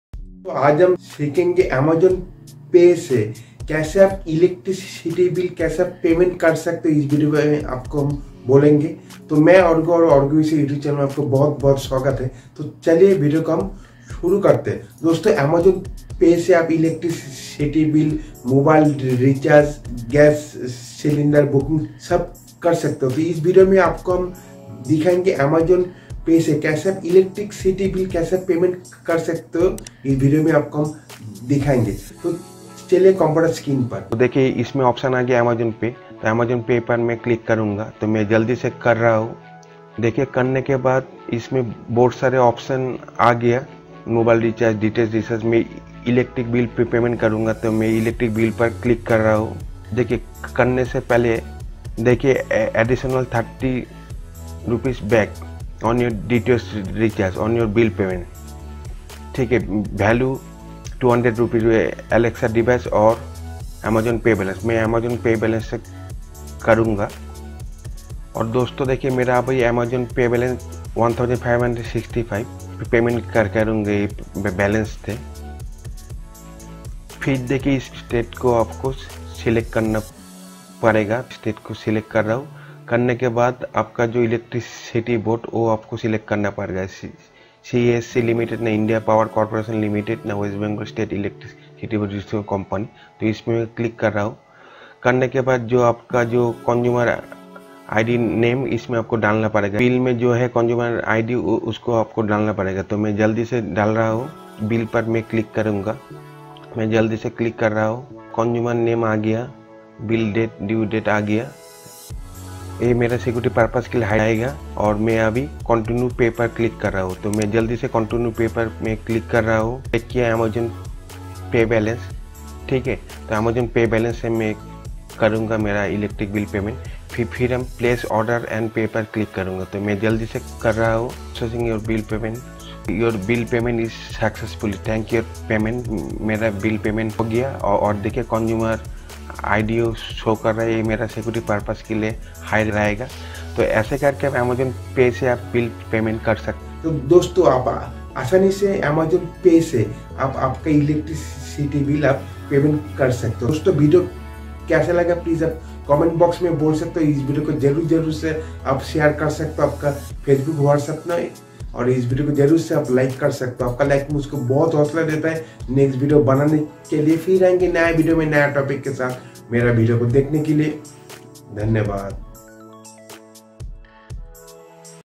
how to pay electricity bill by amazon pay | amazon pay se electricity bill kaise pay kare
Hello. Friend in this video i show you how to pay electricity bill amazon pay.amazon pay se electricity bill kaise pay kare.